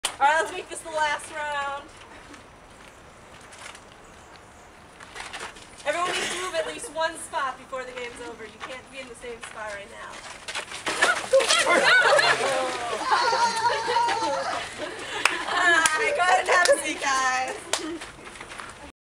Fall 2011 BEETS-Ice Breaker_MVI_1417.MOV
Make eye contact with one other person in the circle and switch without person in the middle seeing you.